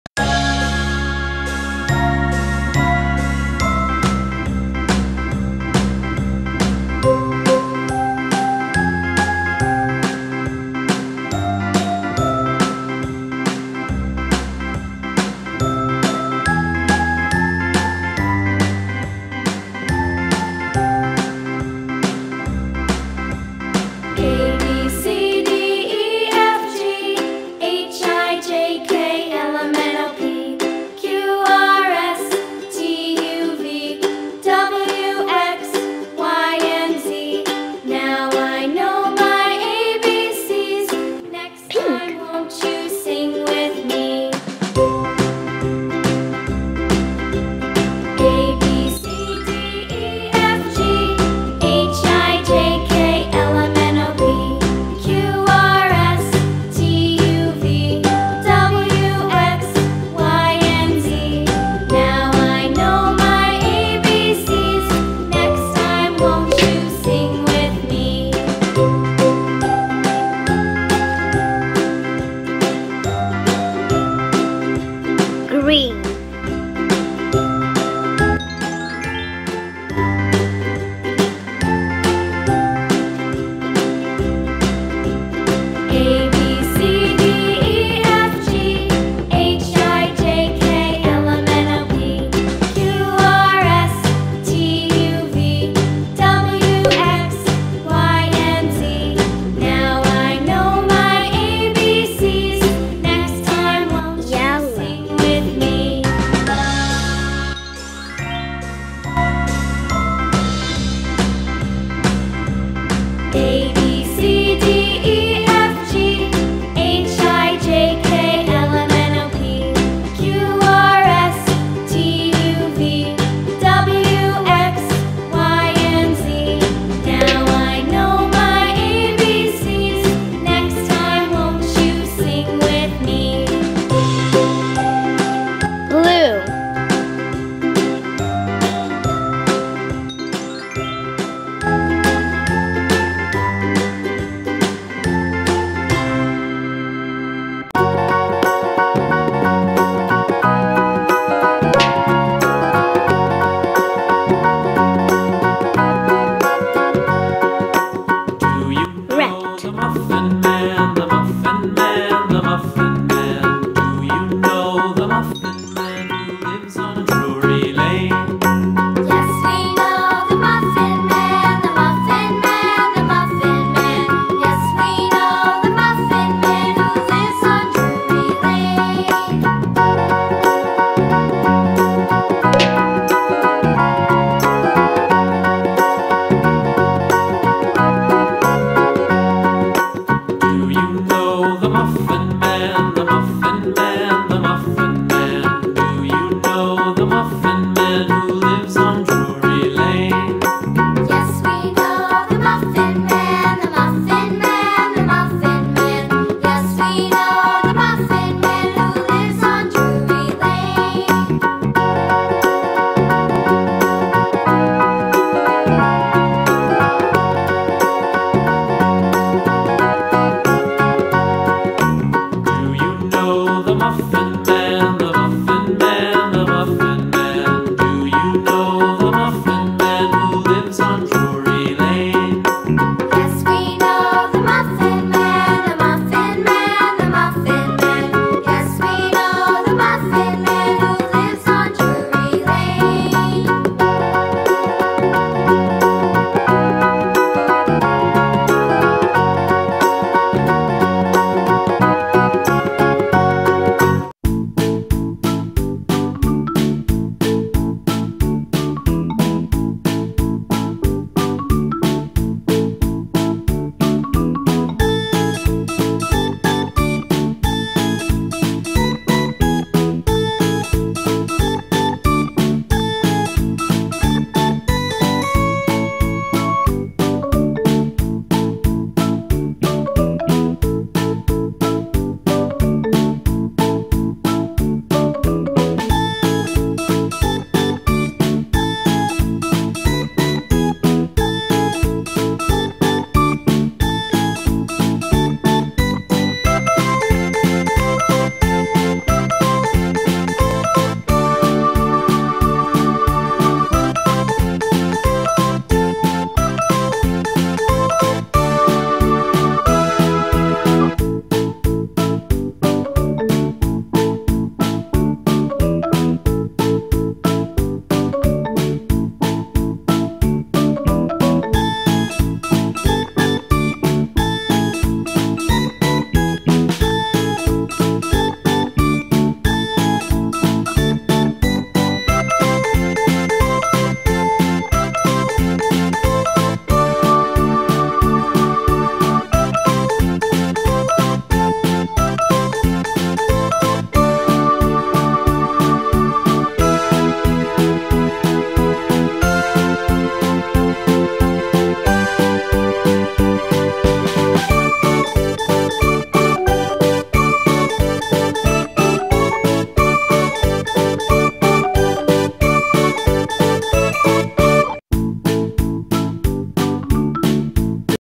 Open Surprise Eggs and find Frozen Elsa and Anna
Learn colors with Surprise eggs with characters of Frozen Elsa, Olaf, Christoff and others. Have fun singing Kids songs!